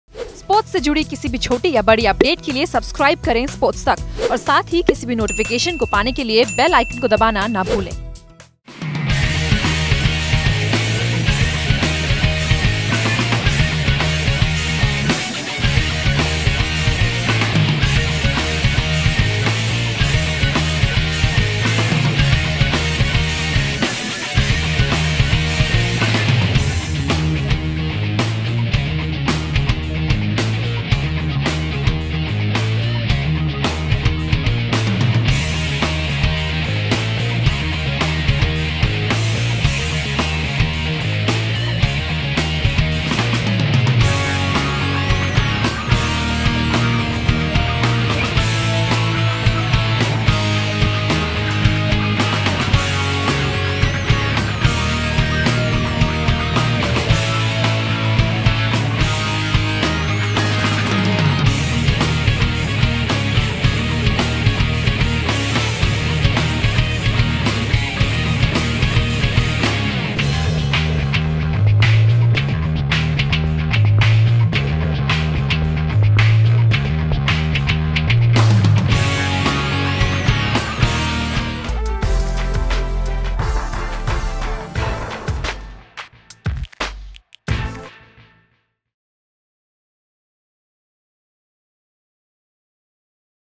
Rajasthan Royals Retain 16 Players | Sports Tak
The 12th edition of the Indian Premier League officially kicked off today with the announcement of player retention from all franchises. Rajasthan Royals, the team that made a strong impact by finishing among the top four teams in the league stage and qualifying for the playoffs, have gone ahead and retained 16 of its players ahead of the IPL auctions for 2019 season.